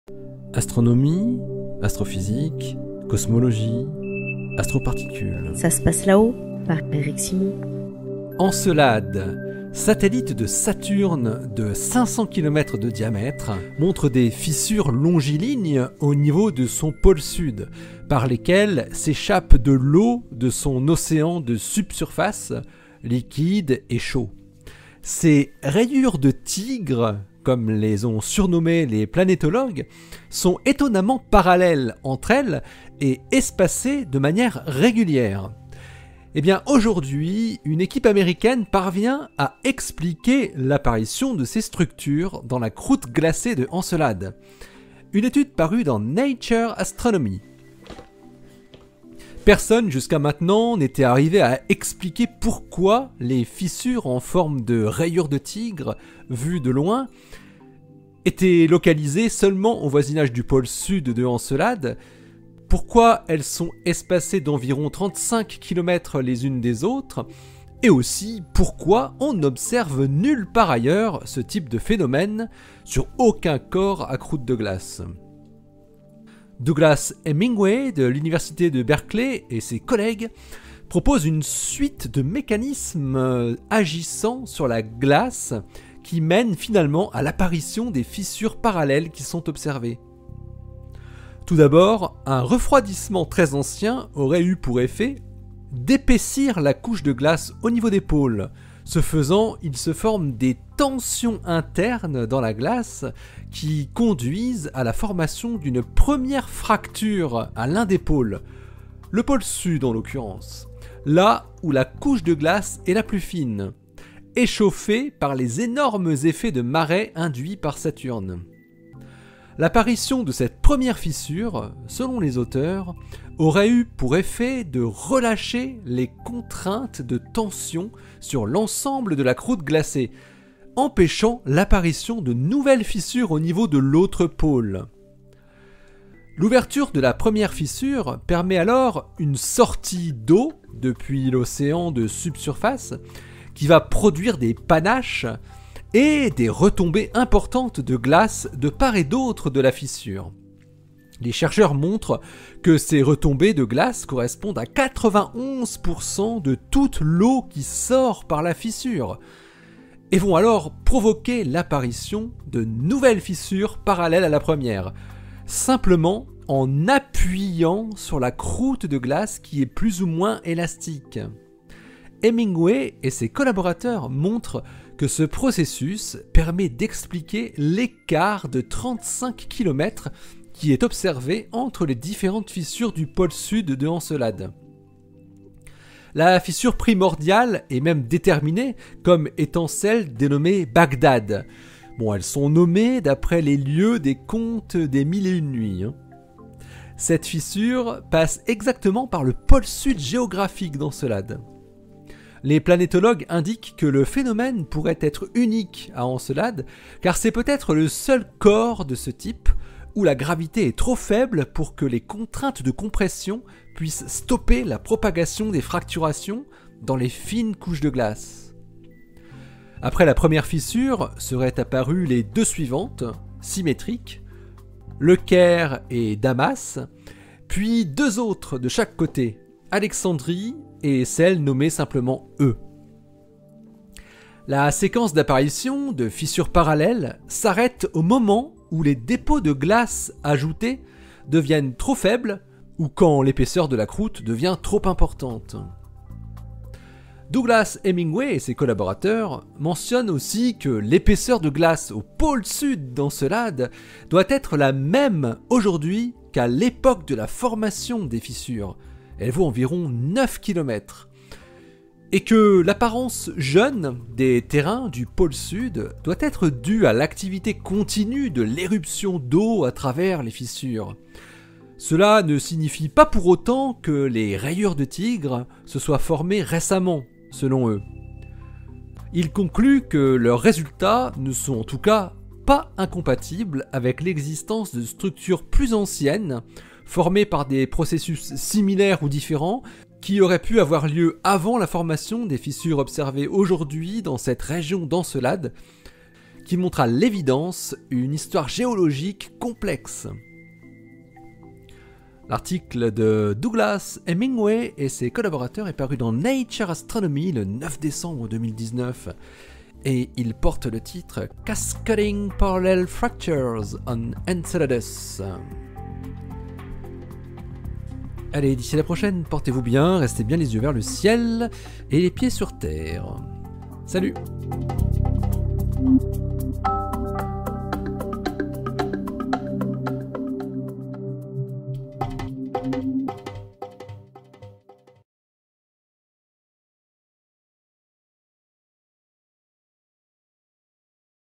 [Astrophysique] Les fissures d'Encelade expliquées
Encelade, satellite de Saturne de 500 km de diamètre, montre des fissures longilignes au niveau de son pôle sud, par lesquelles s’échappe de l’eau de son océan de subsurface, liquide et chaud. Ces « rayures de tigre » comme les ont surnommées les planétologues sont étonnamment parallèles entre elles et espacées de manière régulière. Aujourd’hui, une équipe américaine parvient à expliquer l’apparition de ces structures dans la croûte glacée de Encelade. Une étude parue dans Nature Astronomy.